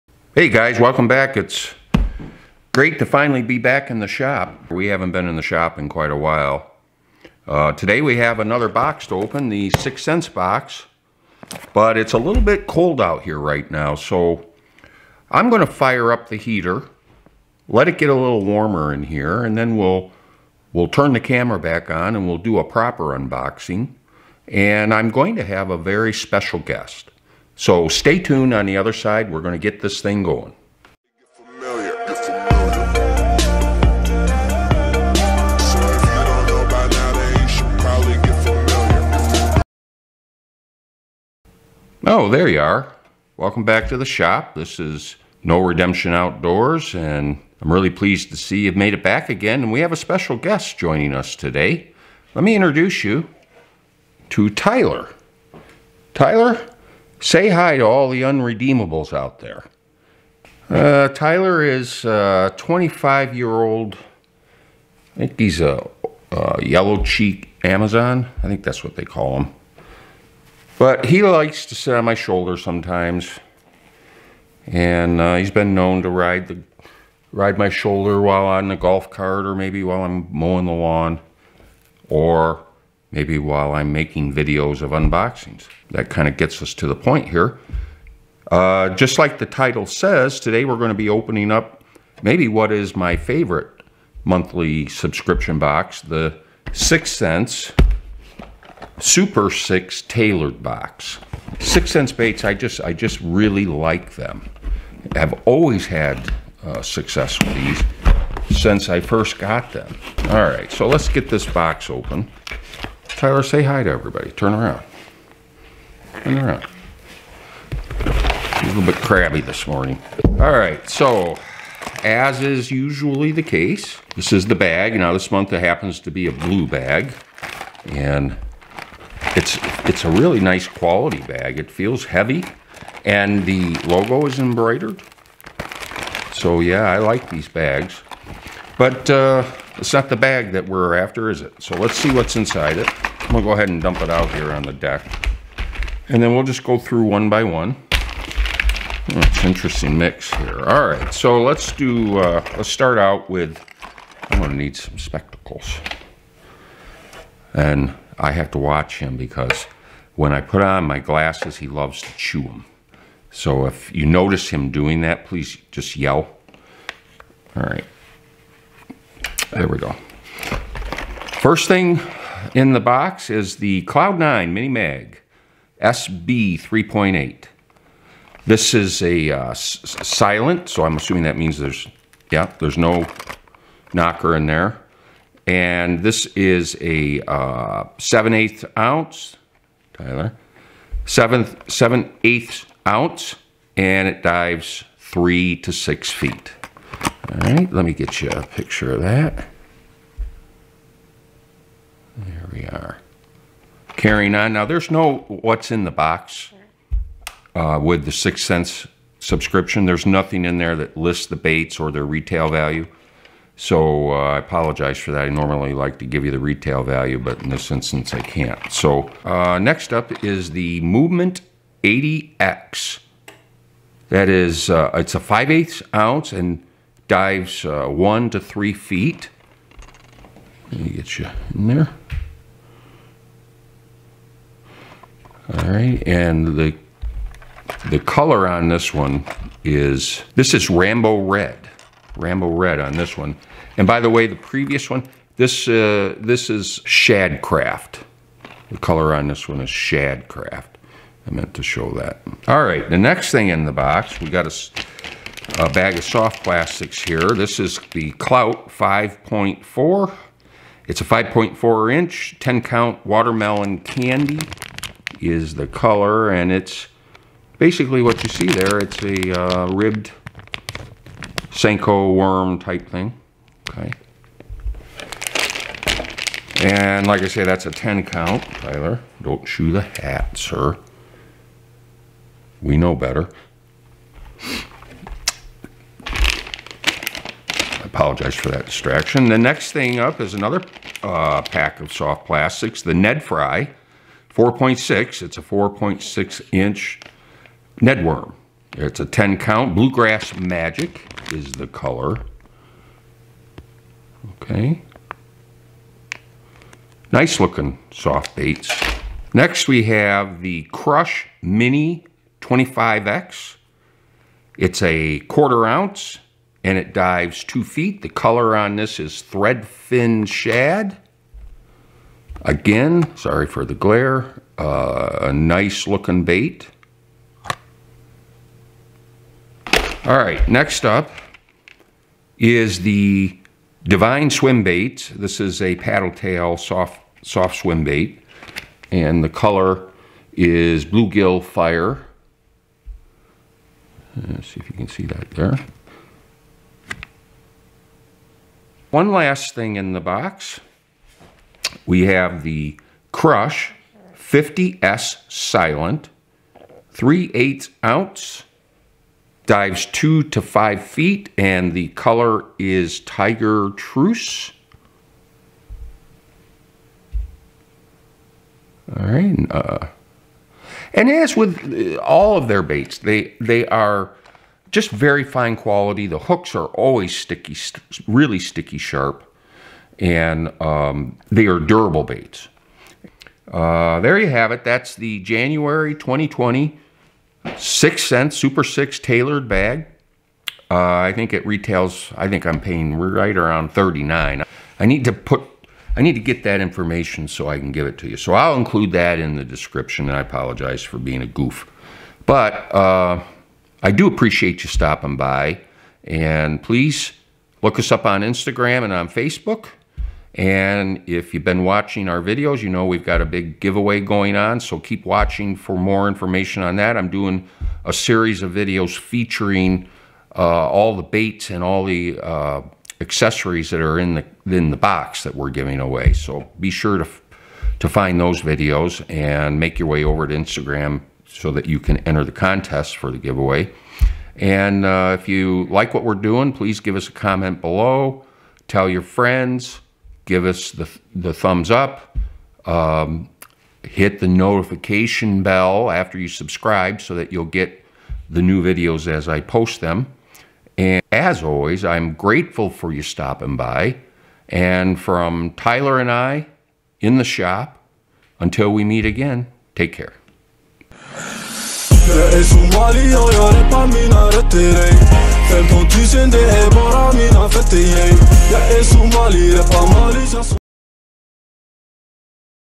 6th Sense Super 6 Tailored Sack ~ January 2020 ~ UNBOXING
Today we open the 6TH SENSE SUPER 6 TAILORED SACK for January 2020.  I'm joined in the shop by my bud, Tyler.  And together we see if this month's box was worth the money.  We rate it a WIN!